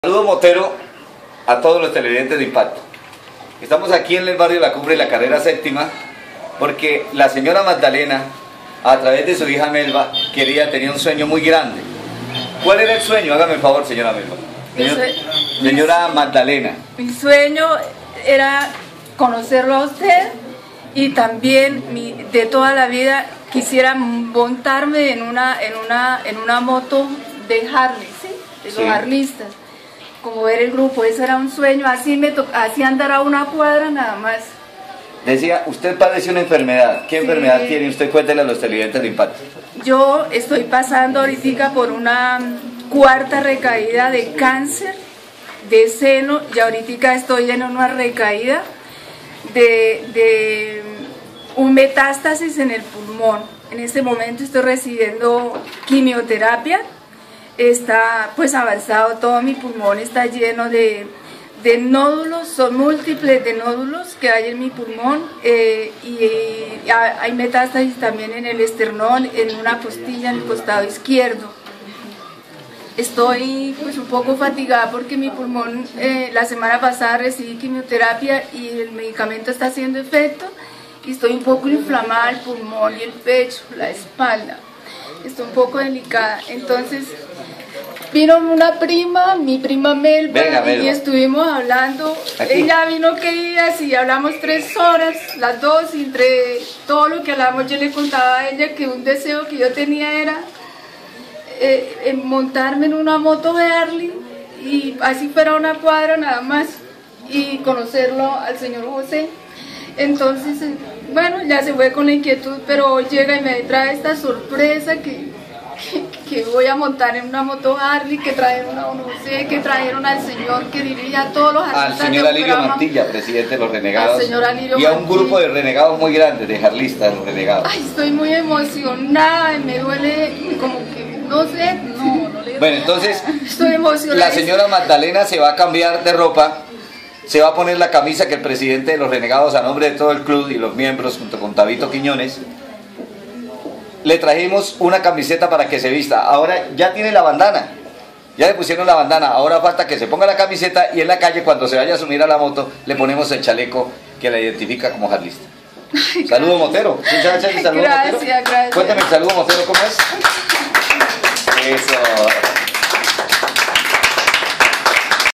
IMPACTO Cumplido Deseo Sra Magdalena conocer al Defensor
Presidente Yiyo Mantilla Presenta Renegados a sra Magdalena